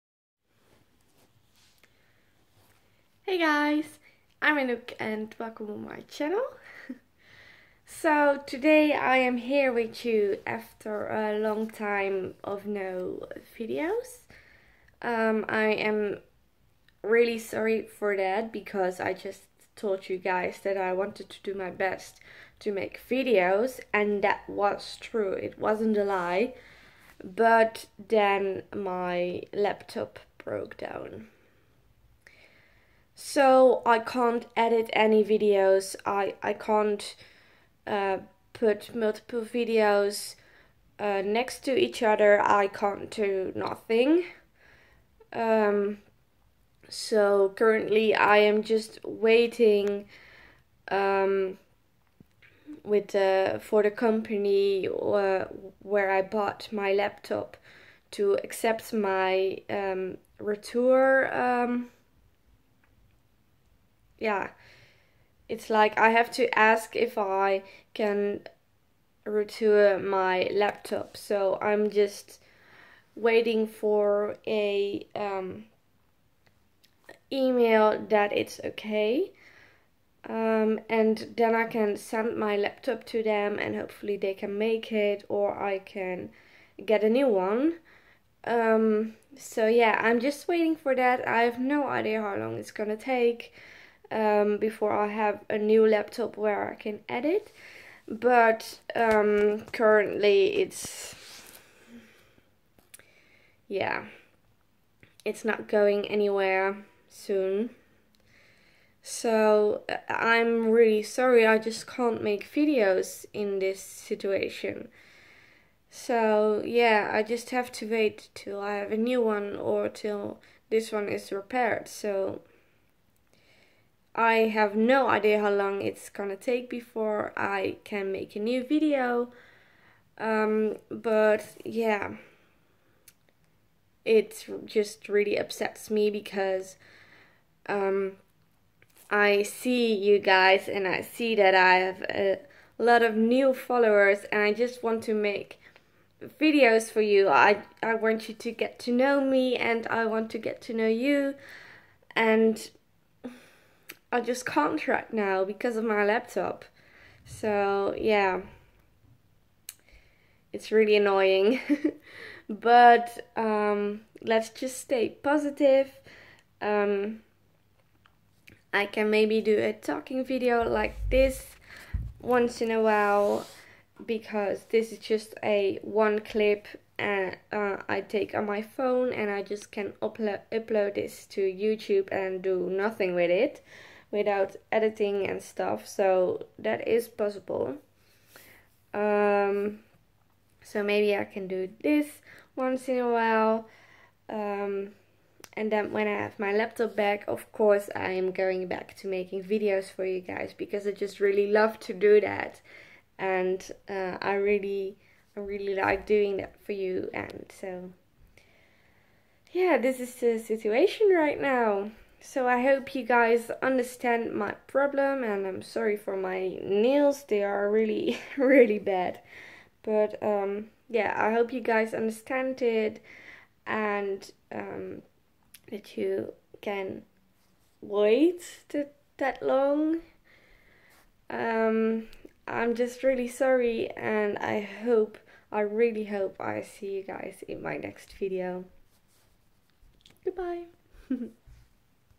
Why I haven't been making video's | I'm sorry
I am so sorry that I haven't been making video's guys. I hope you all understand after watching this video and I hooe I still see you in my next video !